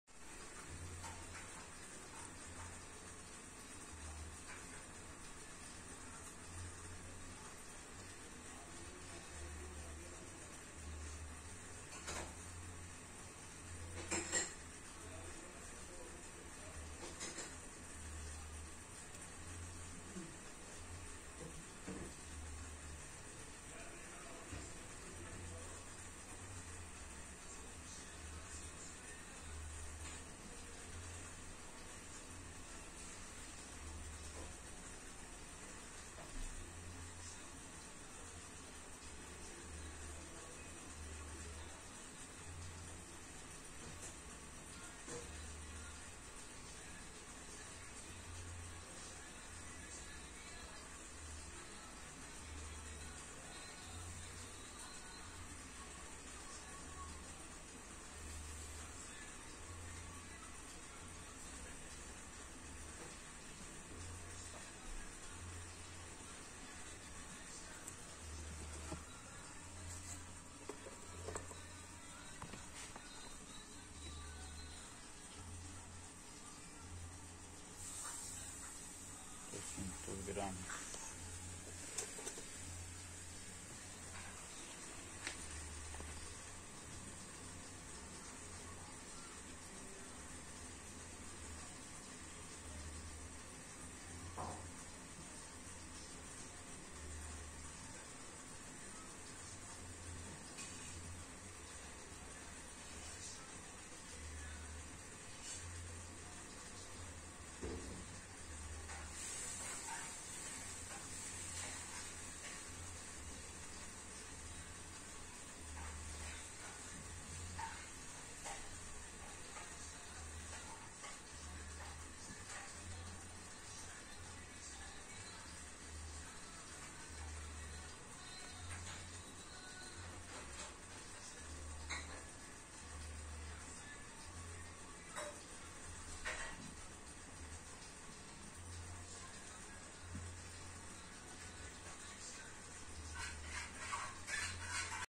encontré un meteorito viejo en descomposicion
rara piedra con peso de más de 200 gramos encontrada en tierras de colombia .